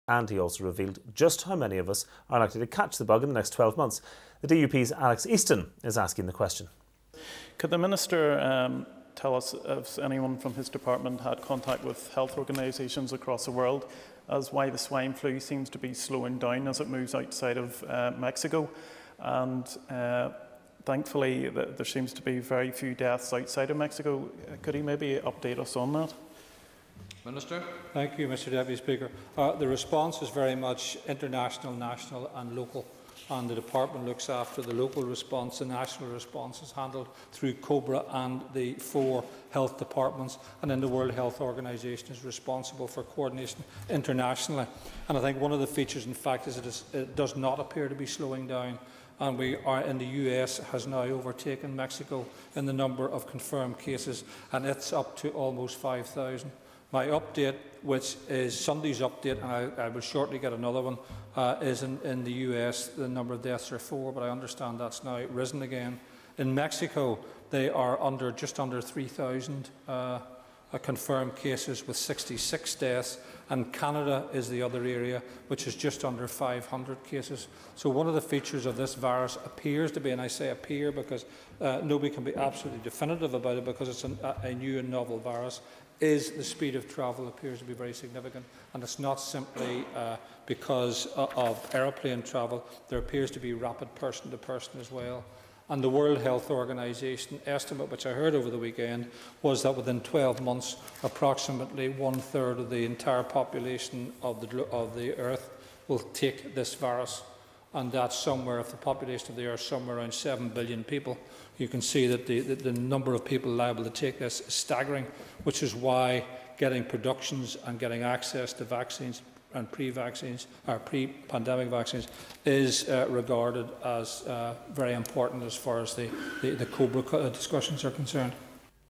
Easton Questions Minister on Swine Flu
DUP North Down MLA Alex Easton questioning the Health Minister about swine flu in Northern Ireland.